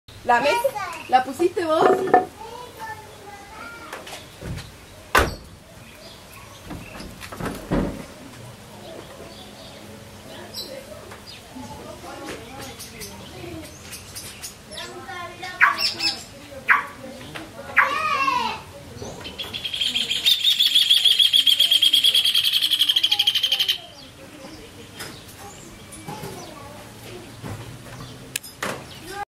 Meliponas Scaptotrigona bipunctata en una casa de Laguna Blanca - Formosa - Argentina
Bellisimo!